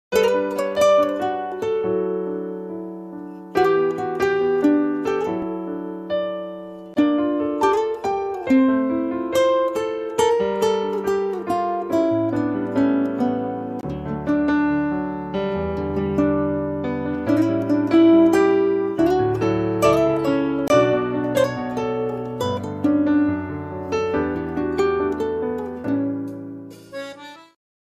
Pomeranian enjoying a chin massage
Video taken by Jeff Graffam